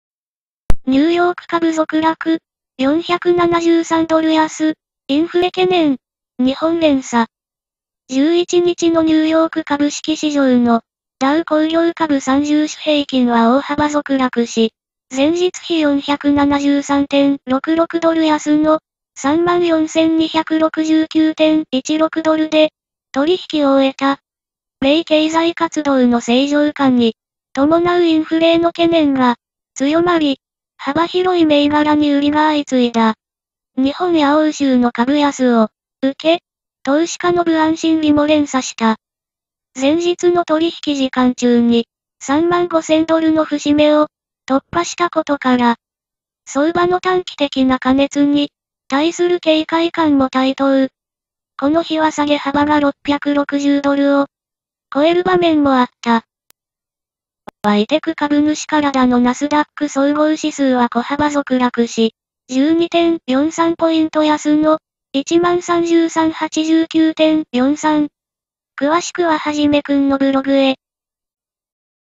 NY株大暴落日本連鎖へ
NY株続落、473ドル安 インフレ懸念、日本連鎖
　11日のニューヨーク株式市場のダウ工業株30種平均は大幅続落し、前日比473.66ドル安の3万4269.16ドルで取引を終えた。米経済活動の正常化に伴うインフレへの懸念が強まり、幅広い銘柄に売りが相次いだ。日本や欧州の株安を受け、投資家の不安心理も連鎖した・・・